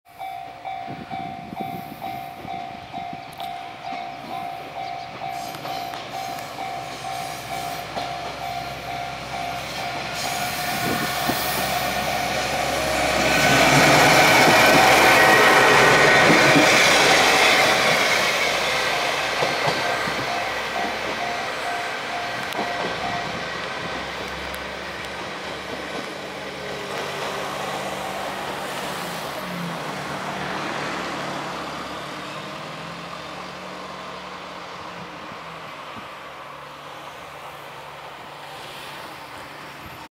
日南線キハ40系 油津駅付近にて
日南線のキハ40系を油津駅付近にて撮影です。